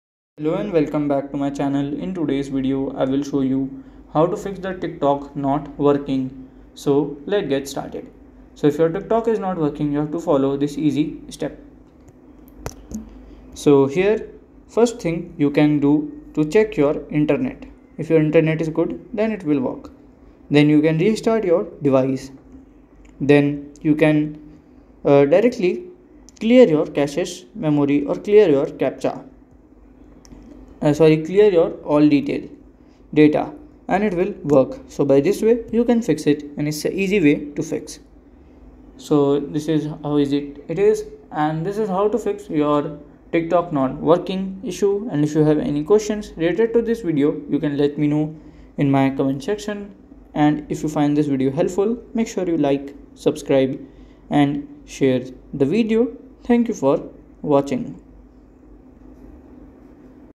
✅ How to fix TikTok Not Working Issue (Full Guide)
Encountering issues with TikTok not working? Learn how to fix TikTok not working issues with our comprehensive guide. In this tutorial, we provide troubleshooting steps to address common problems that users may encounter while using the TikTok app, ensuring a seamless and enjoyable experience on the platform. Whether you're facing login issues, video playback problems, or app crashes, we'll walk you through the process of resolving the issue and getting back to using TikTok smoothly. Watch this video to troubleshoot TikTok not working issues and continue creating and sharing content hassle-free.

Key Learning Points:
- Step-by-step troubleshooting guide to fixing TikTok not working issues
- Addressing common problems such as login issues, video playback problems, and app crashes
- Tips for ensuring a seamless and enjoyable experience on TikTok